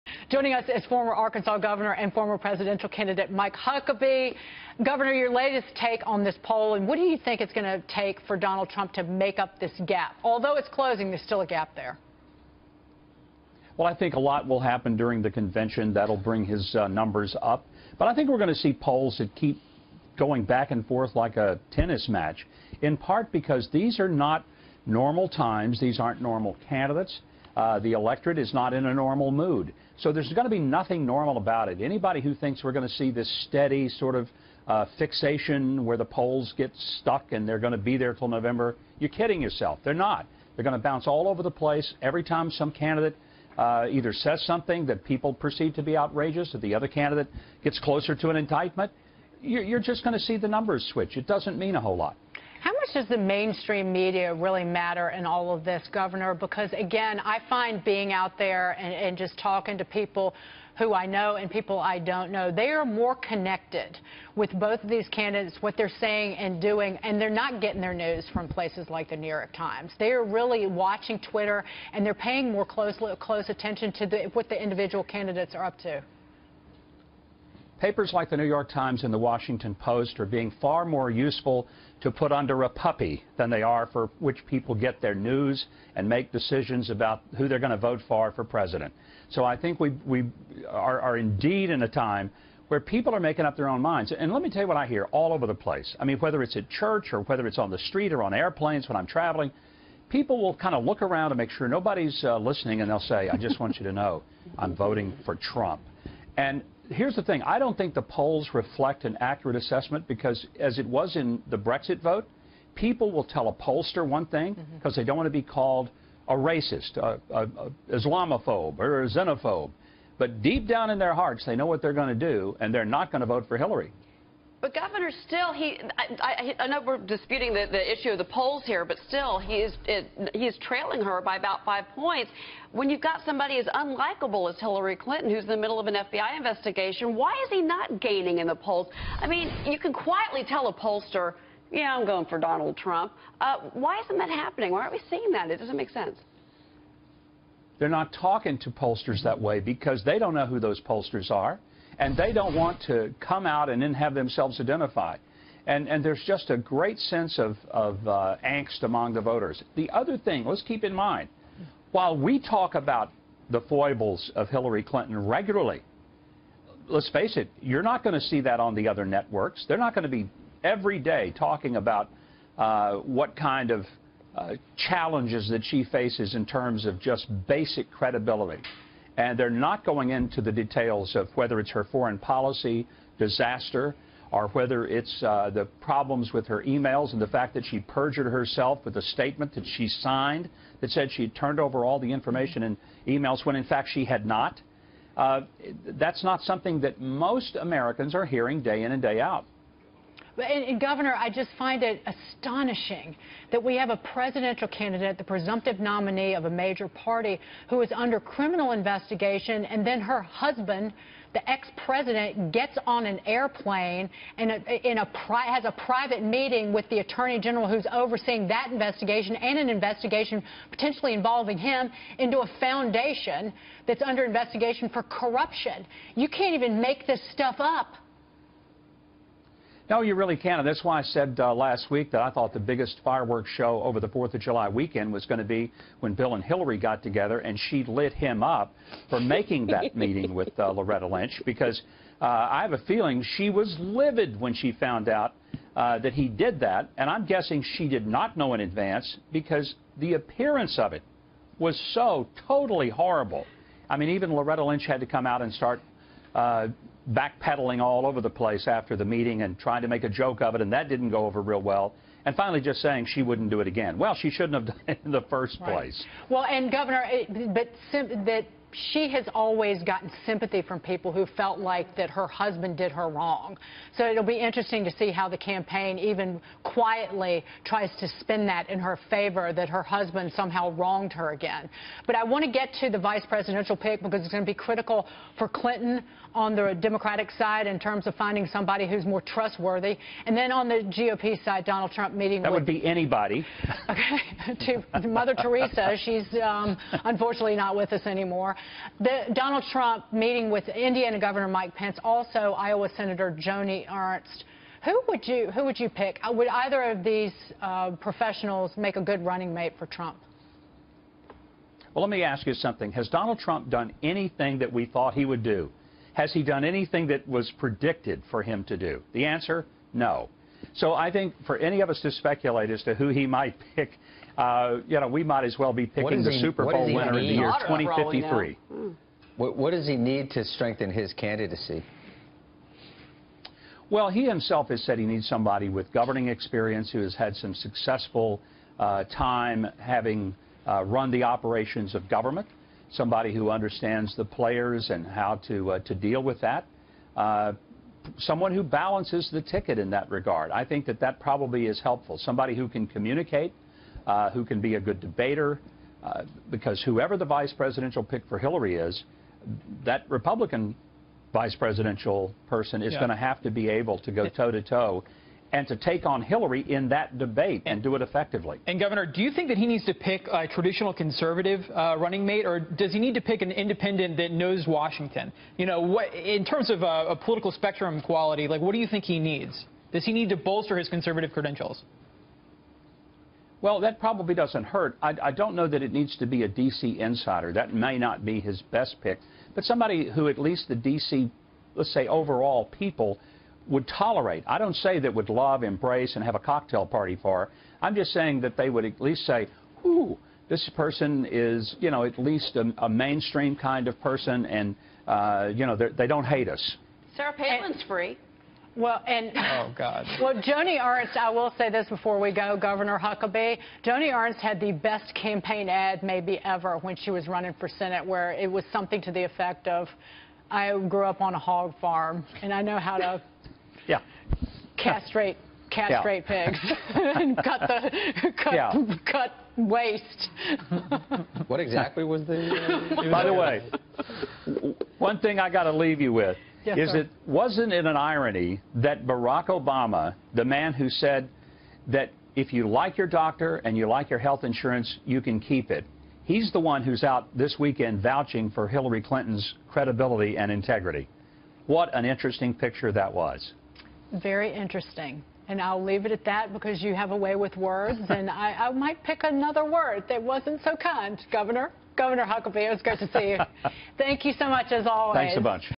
Does Trump need a traditional conservative as V.P.?
Former Republican presidential candidate Mike Huckabee on the 2016 presidential race.